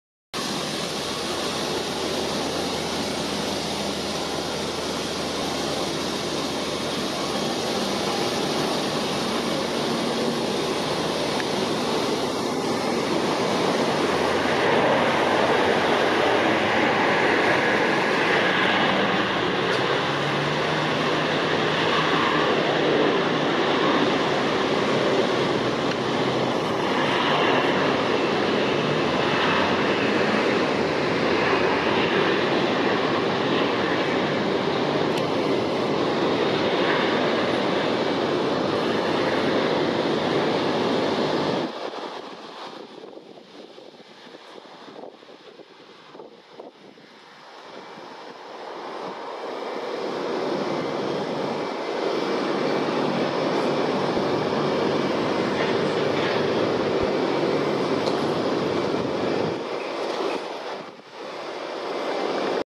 Auckland Airport | Air New Zealand | Airbus A320-232 (ZK-OXA) landing RWY23L
credits---James Raven